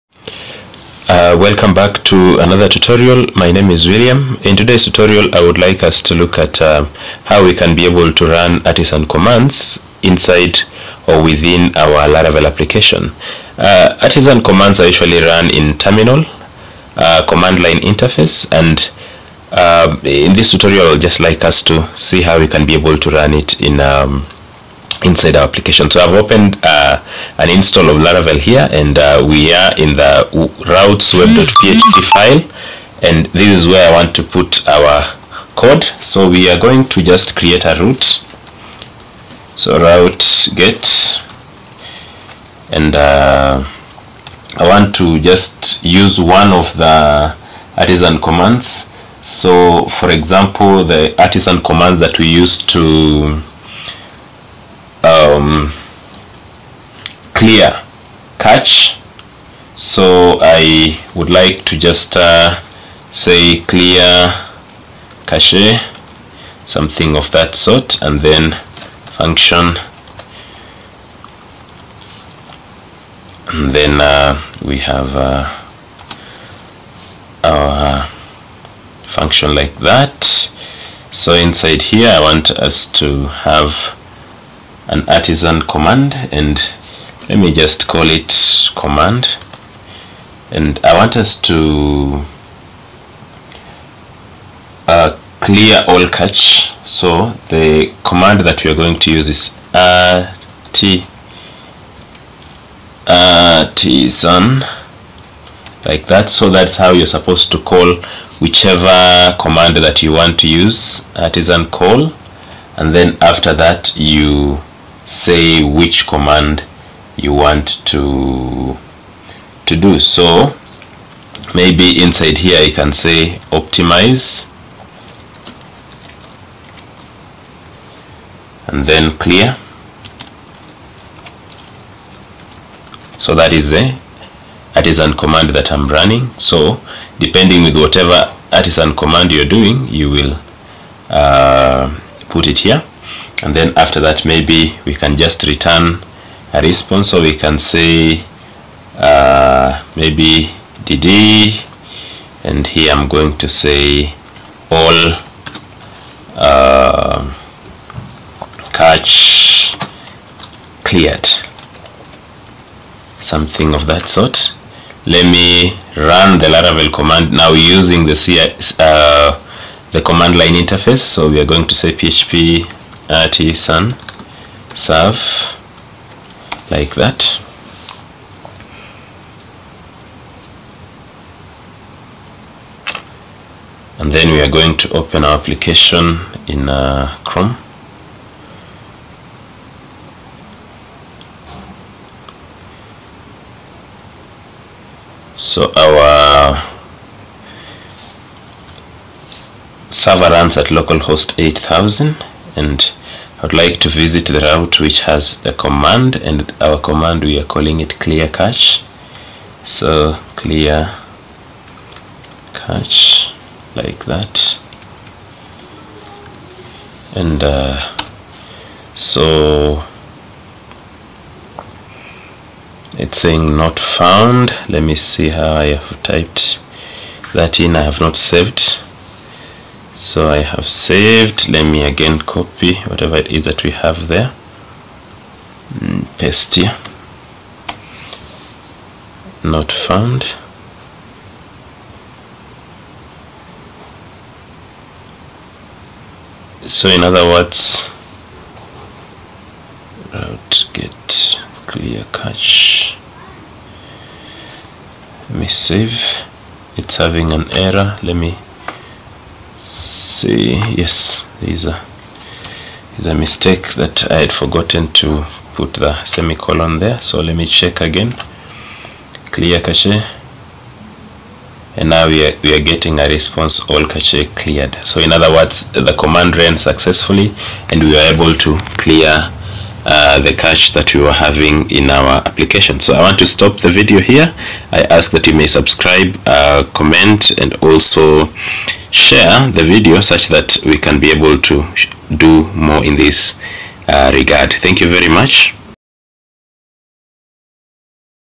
Run artisan commands directly in a Laravel project
Learn how to run Laravel Artisan commands inside Laravel application. This is useful especially for users who have their application hosted in shared hosting and are not allowed to run commands on their hosting plan.